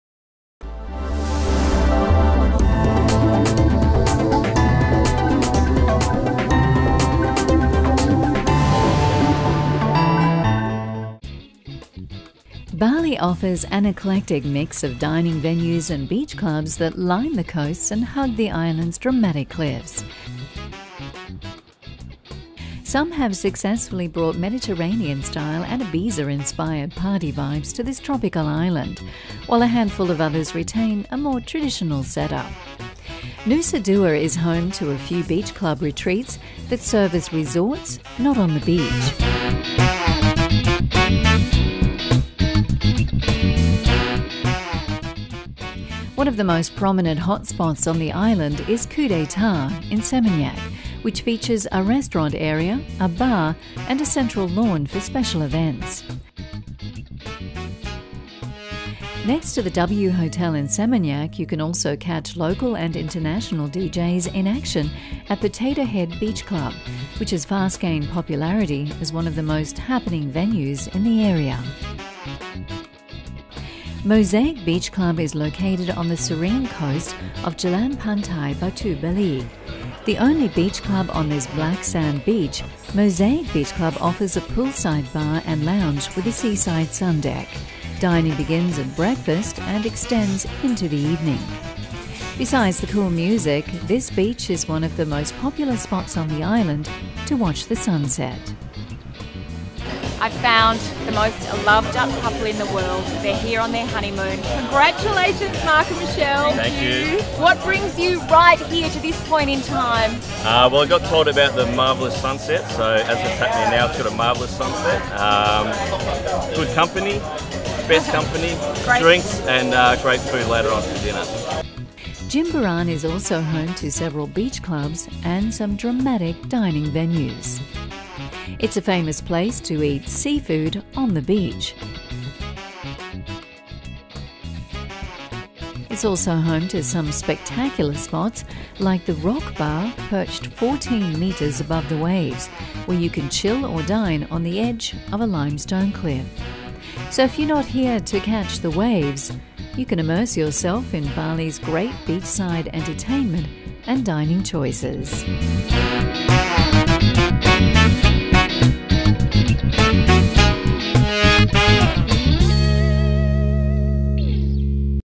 Luxury Escapes - Top Beach Clubs in Bali
Bali offers an eclectic mix of dining venues and beach clubs that line the coast that hug the island's dramatic cliffs. Some have successfully brought Mediterranean style and Ibiza inspired party vibes to this tropical island while a handful others retain a more traditional setup